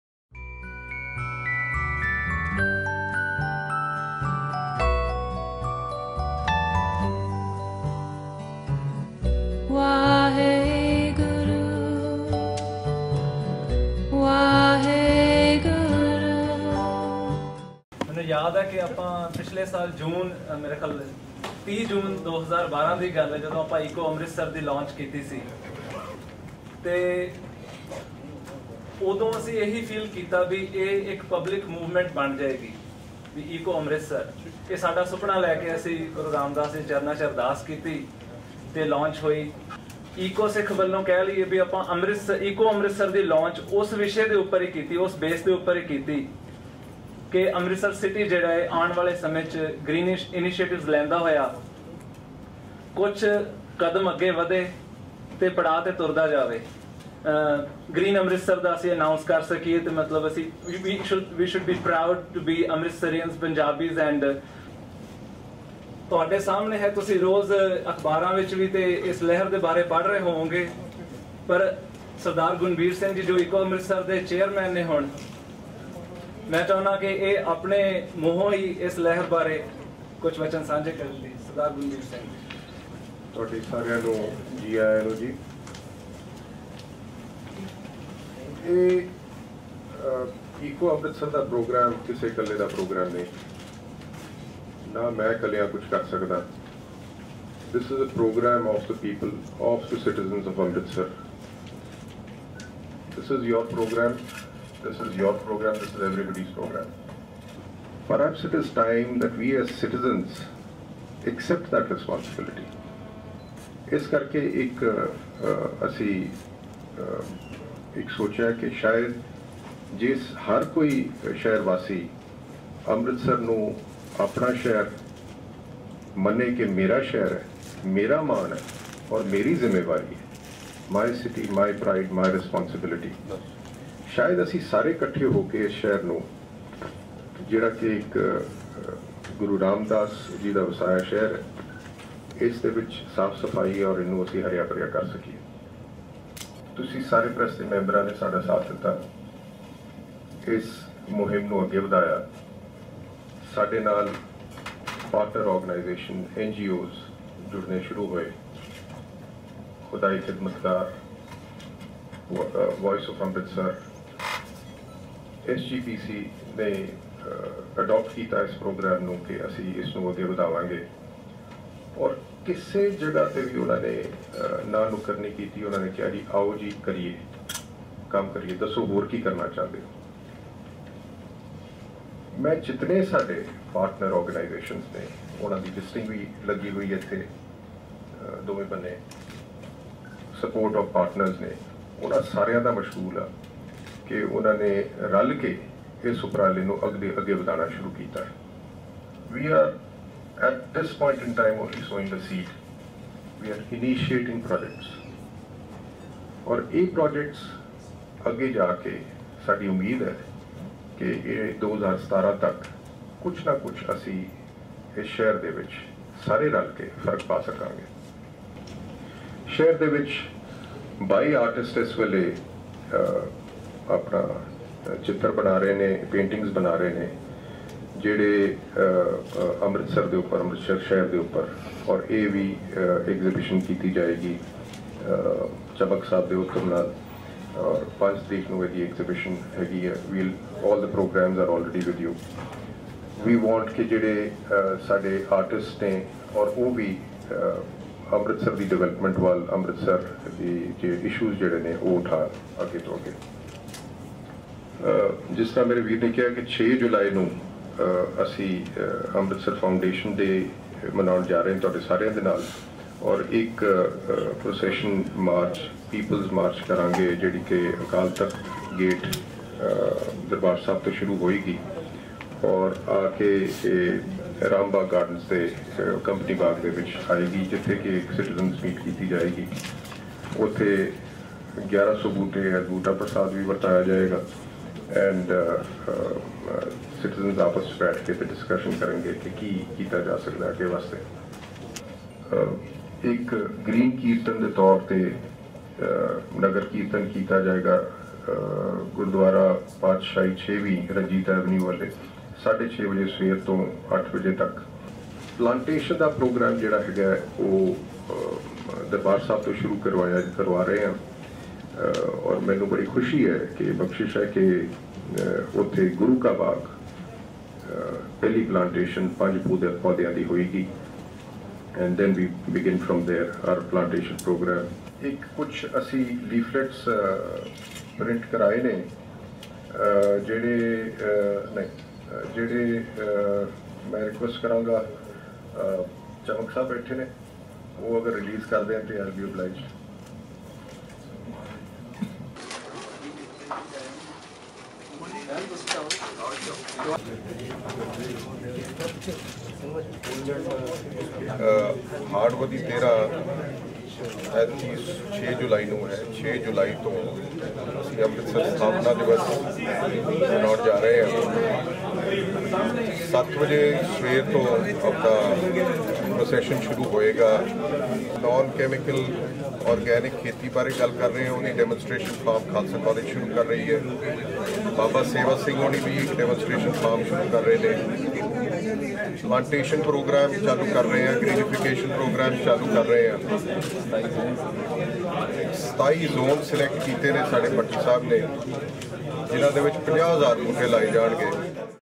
Eco-Amritsar Press Conference
A press conference was organised on the 4th of July 2013 to apprise the press and the stakeholders about the programs decided to celebrate Amritsar Foundation Day 2013